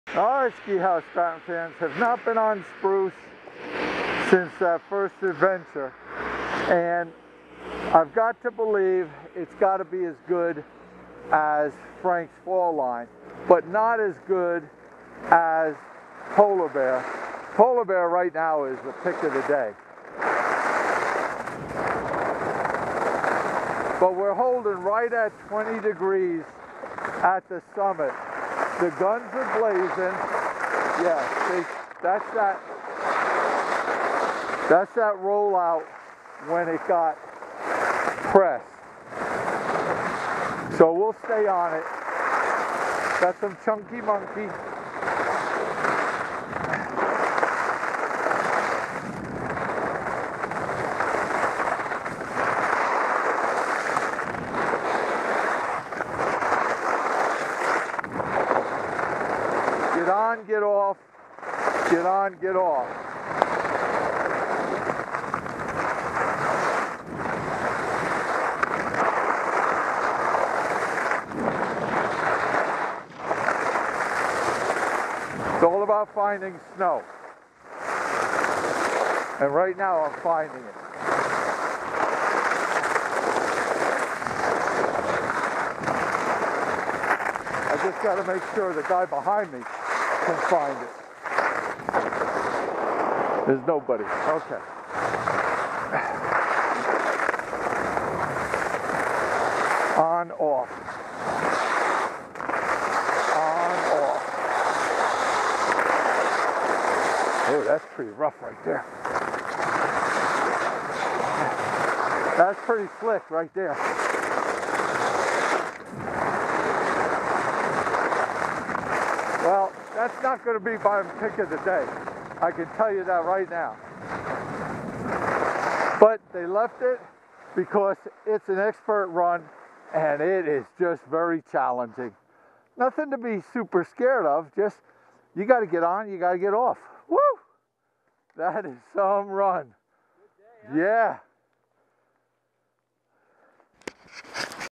12/02/22 The Big Man stepping it up on Upper Spruce.
That will get your heart beating.